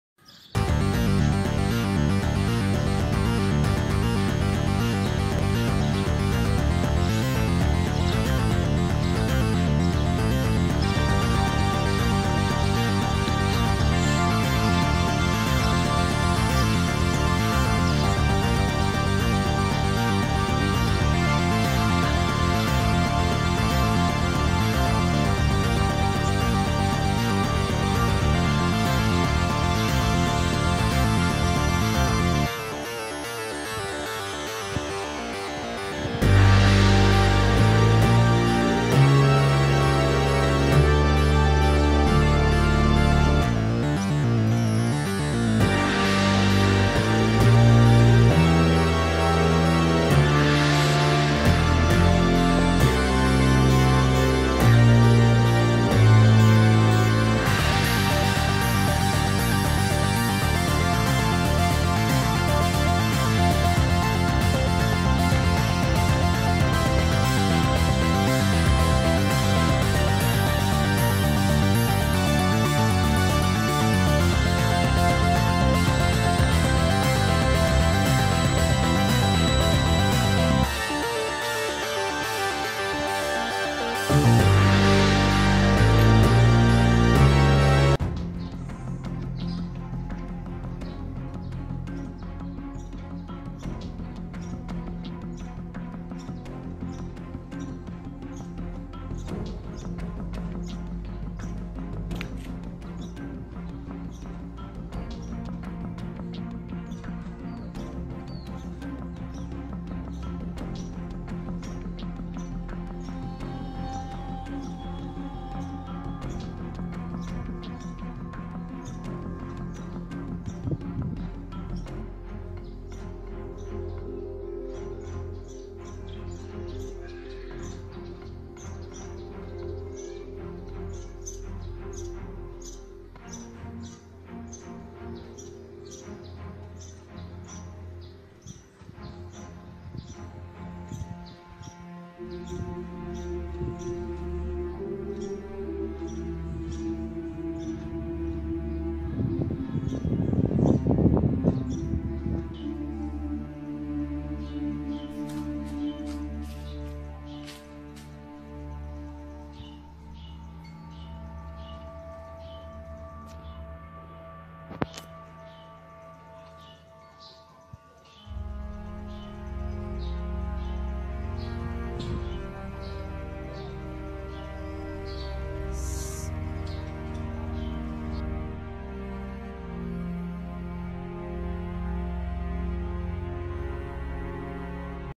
Pekerjaan Beresiko Tinggi: Kabel Sutet
Menaiki Kabel Suttet adalah pekerjaan yang sangat beresiko Tinggi, Pekerjaan ini membutuhkan keberanian yang sangat kuat. karena salah melangkah atau terpelesat nyawa taruhannya.